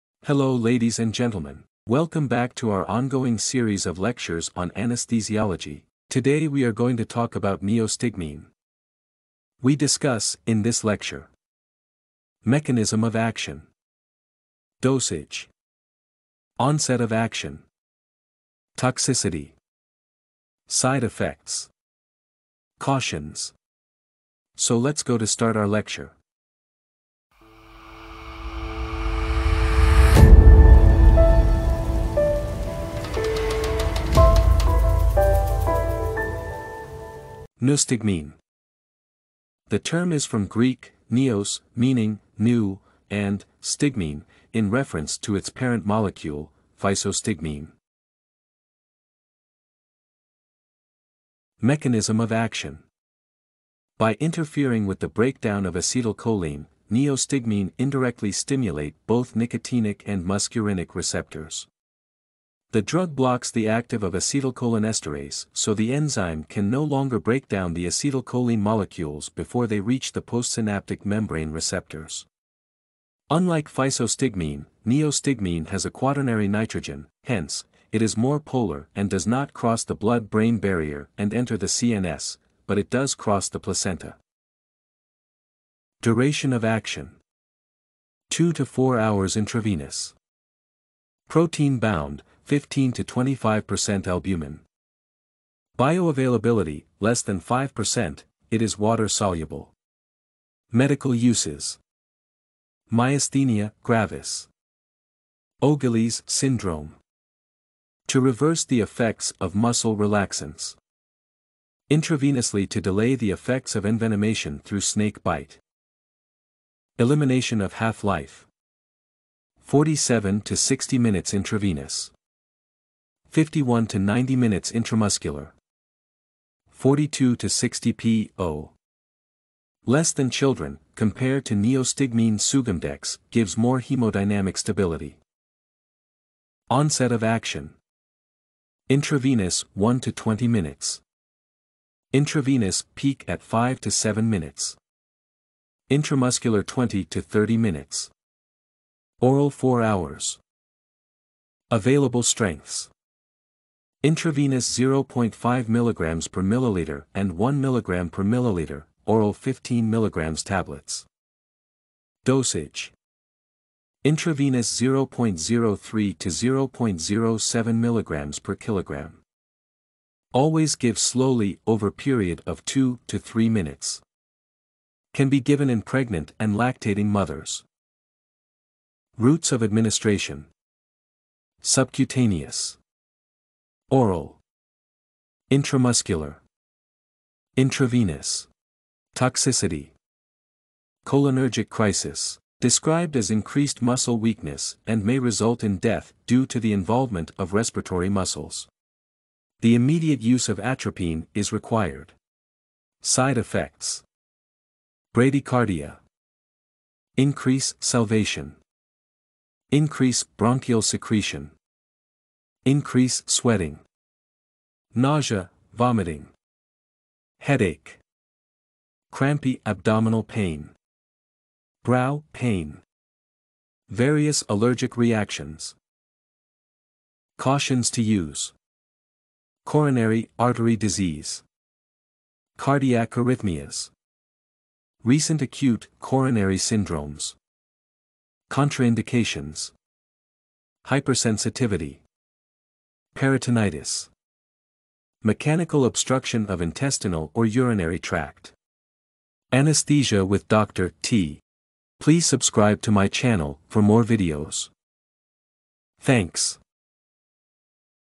Neostigmine | mechanism of action | side effects
Neostigmine.
The term is from Greek 'neos' meaning 'new' and stigmine in reference to its parent molecule Physostigmine.
During of action.
Medical uses.
Elimination of half life.
Routes of administration.
Onset of action.
Dose.
Toxicity.
Side effects.
Contraindications.
Caution to use.

For more videos visit to below links ☺️☺️

                     THANKS 👍👍